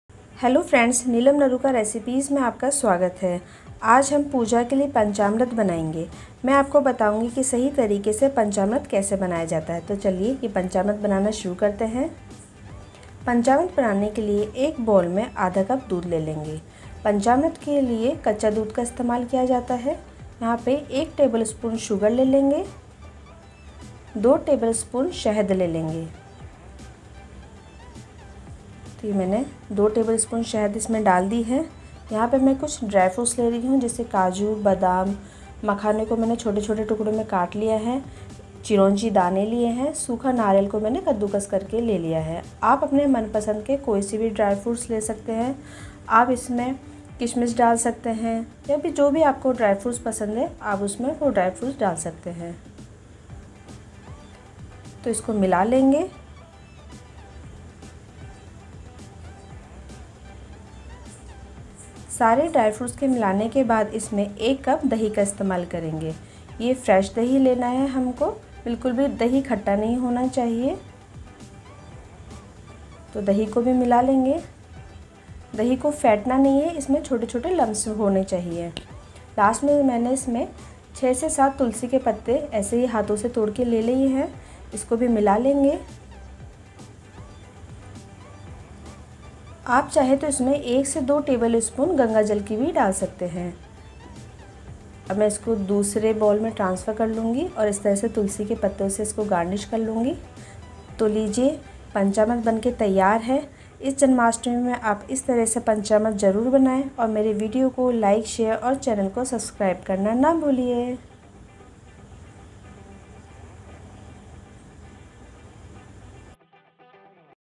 Panchamrut Recipe | पंचामृत बनाने का सही तरीका | Janmastami Special Panchamrit Recipe | NeelamNaruka
Ingredients:-
Raw Milk - 1/2 Cup
Fresh Curd -  1 Cup
Tulsi Leaves -  7